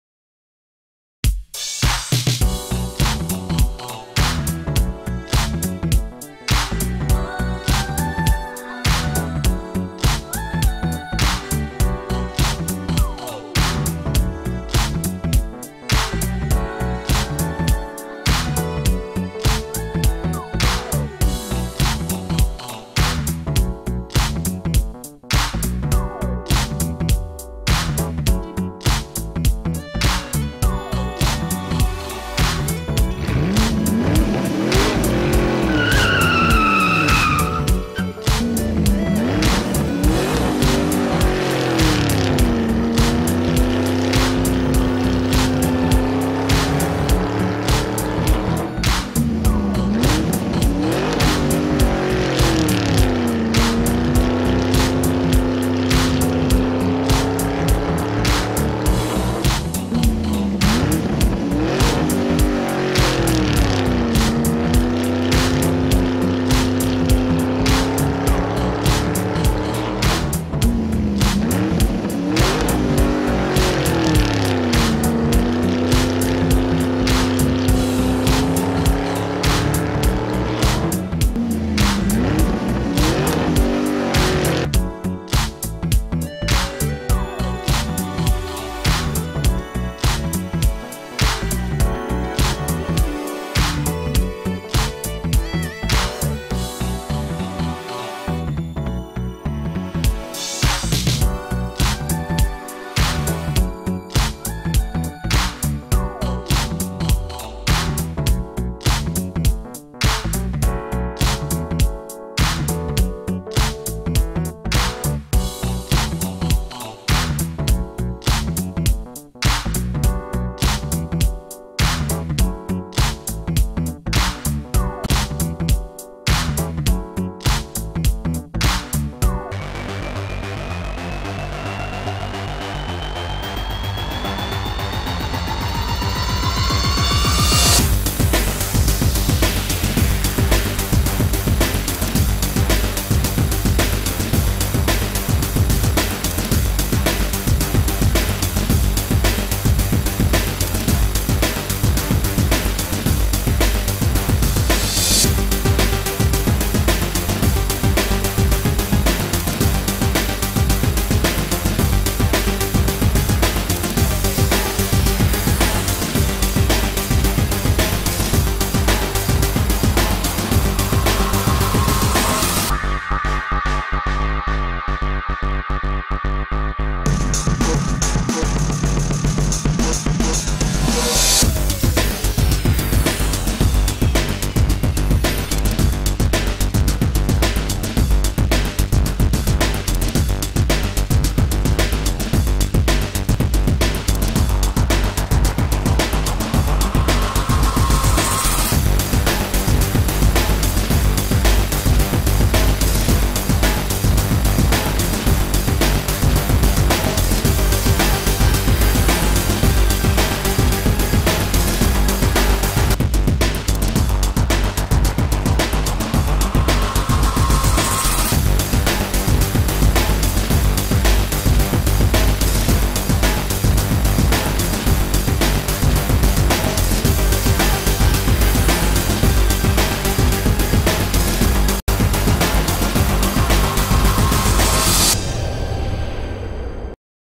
Amazing chain reaction domino effect Jeep car in fire track
Amazing experiment, after assembly of the track, the car takes some rounds, then all the track is surrounded by safety matches to look like a fire track.
the car itself started the fire cascade.